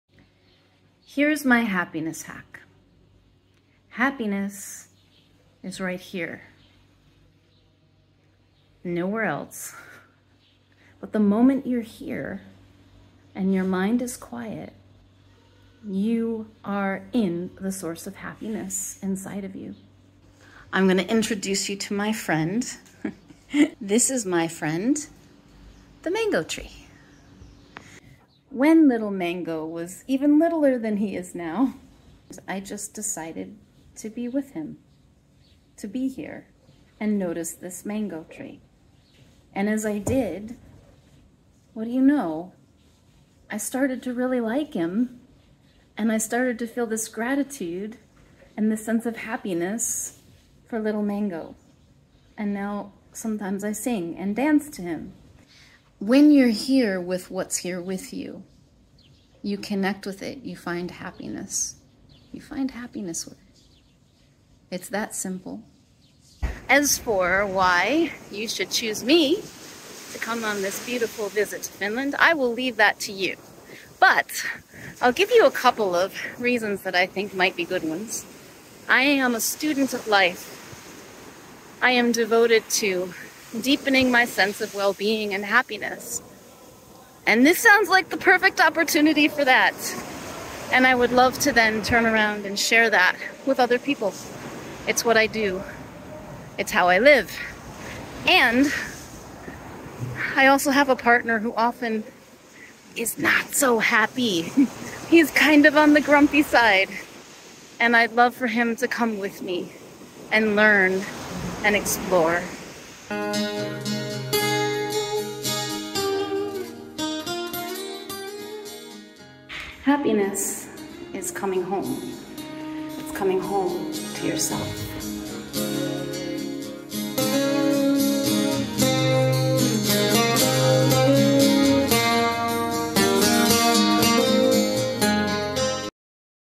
How to Find Happiness HappinessHack (73 of 100)
Where can you find happiness? Here's the simple truth and my HelsinkiHappinessHacks for VisitFinland @ourfinland.